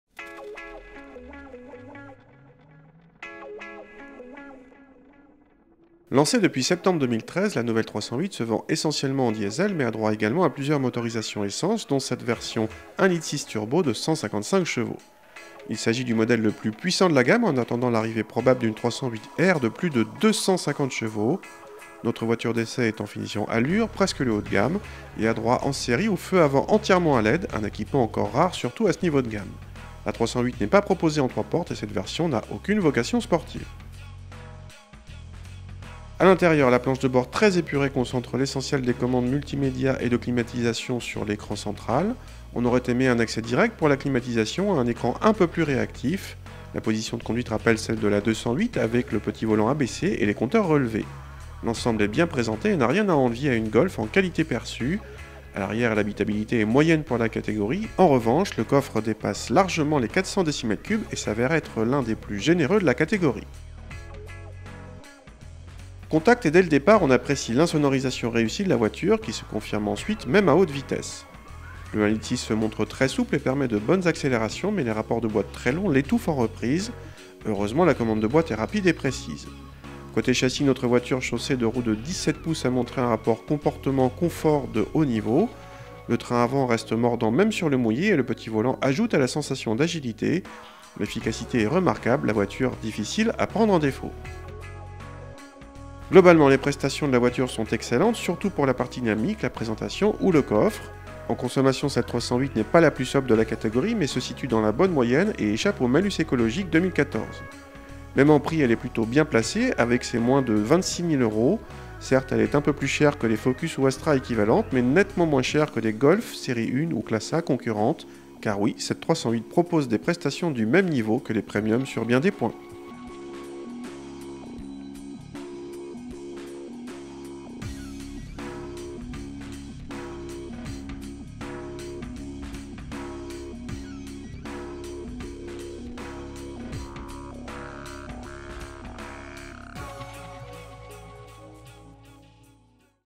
Essai Peugeot 308 1.6 THP 155 Allure 2014
Lancée depuis septembre 2013, la nouvelle 308 se vend essentiellement en diesel mais a droit également à plusieurs motorisations essence, dont cette version 1.6 l turbo de 155 chevaux. Il s'agit du modèle le plus puissant de la gamme en attendant l'arrivée probable d'une 308 R de plus de 250 ch ! Notre voiture d'essai est en finition Allure, presque le haut de gamme, et a droit en série aux feux avant entièrement à LED, un équipement encore rare surtout à ce niveau de gamme. La 308 n'est pas proposée en 3 portes et cette version n'a aucune vocation sportive.

A l'intérieur, la planche de bord très épurée concentre l'essentiel des commandes multimédia et de climatisation sur l'écran central. On aurait aimé un accès direct pour la climatisation et un écran un peu plus réactif. La position de conduite rappelle celle de la 208 avec le petit volant abaissé et les compteurs relevés. L'ensemble est bien présenté et n'a rien à envier à une Golf en qualité perçue. A l'arrière, l'habitabilité est moyenne pour la catégorie. En revanche, le coffre dépasse largement les 400 dm3 et s'avère être l'un des plus généreux de la catégorie.

Contact et dès le départ on apprécie l'insonorisation réussie de la voiture, qui se confirme ensuite même à haute vitesse. Le 1.6 l se montre très souple et permet de bonnes accélérations mais les rapports de boîte très longs l'étouffent en reprises. Heureusement la commande de boîte est rapide et précise. Côté châssis, notre voiture chaussée de roues de 17 pouces a montré un rapport comportement/confort de haut niveau. Le train avant reste mordant même sur le mouillé et le petit volant ajoute à la sensation d'agilité. L'efficacité est remarquable, la voiture difficile à prendre en défaut.

Globalement, les prestations de la voiture sont excellentes, surtout pour la partie dynamique, la présentation ou le coffre. En consommations, cette 308 n'est pas la plus sobre de la catégorie, mais se situe dans la bonne moyenne et échappe au malus écologique 2014. Même en prix, elle est plutôt bien placée avec ses moins de 26 000 €. Certes, elle est plus chère que les Focus ou Astra équivalentes, mais nettement moins que les Golf, Série 1 ou Classe A  concurrentes. Car oui, cette 308 propose des prestations du même niveau que les premium sur bien des points.